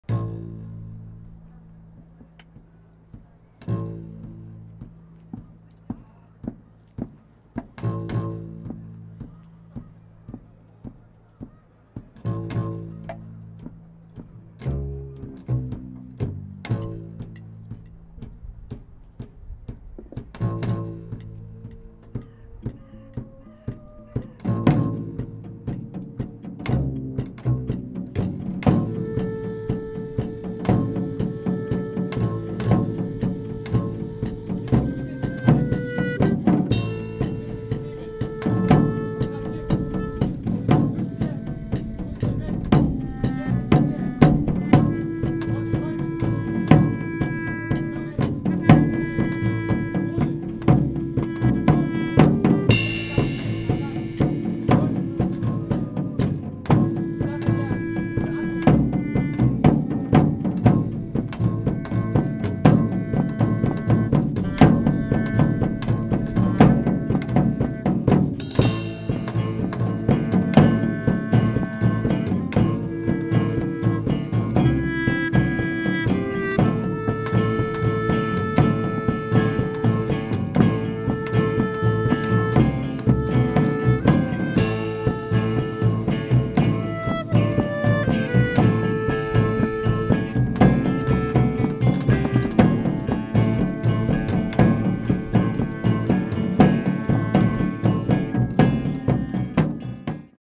Roots of Trust / Timelapse final performance
Roots of Trust Dortmund, April 30th / May 4th 2018 KJT Theater Dortmund (Germany) with Transplanisphere (Paris, France) Teatro Rigodon (Rieti, Italy) Ex Quorum (Evora, Portugal) supported by Erasmus+ Youth Exchange and OFAJ/DFJW